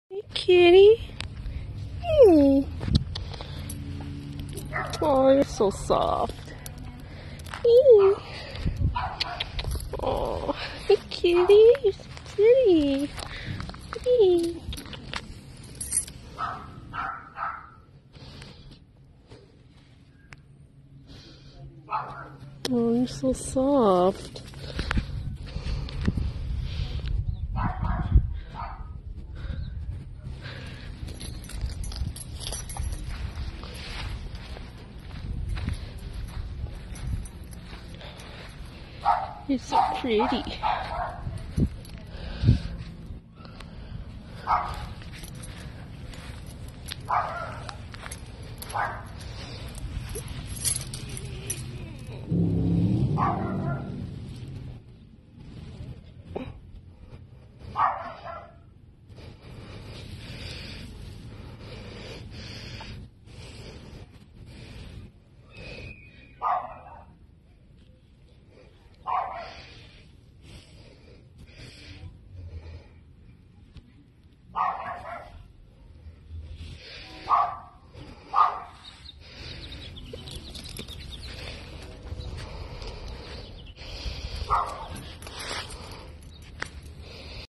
Affectionate gray tabby cat
Random affectionate gray tabby cat I encountered during a walk